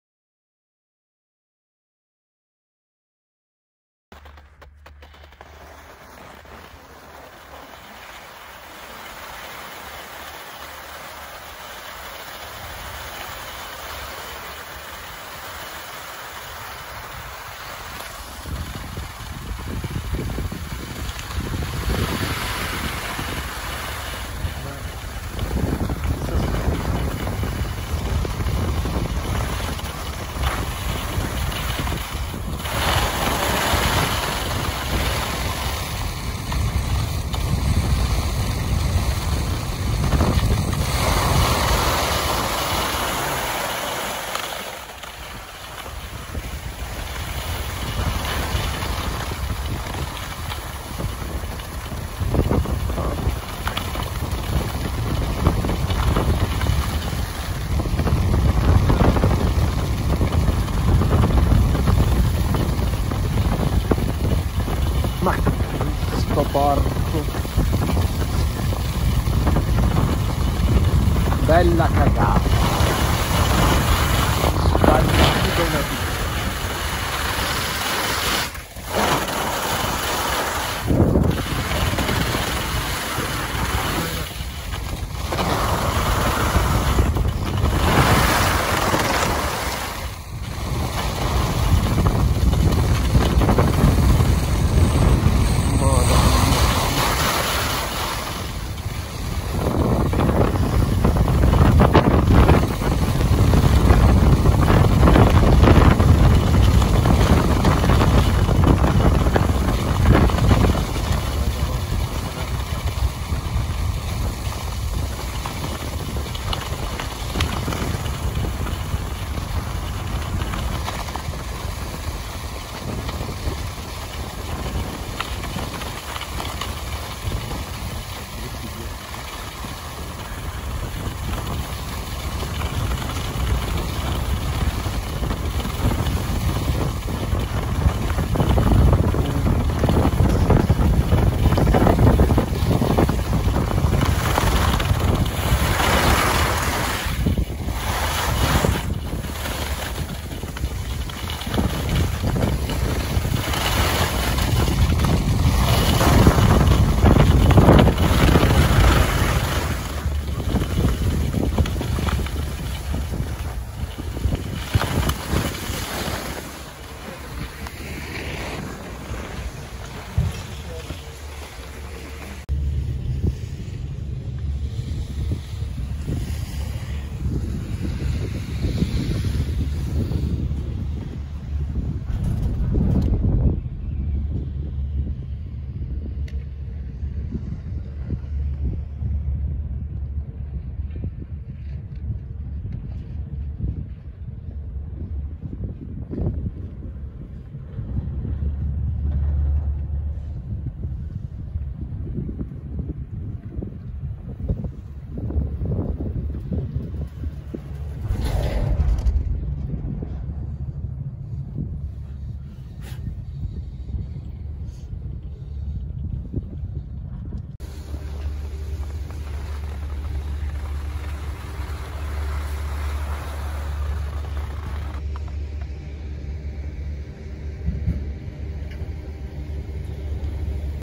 PISTA 11 ORSO BRUNO a Marilleva
Ripresa al mattino presto a Dicembre 2019 con una neve splendida.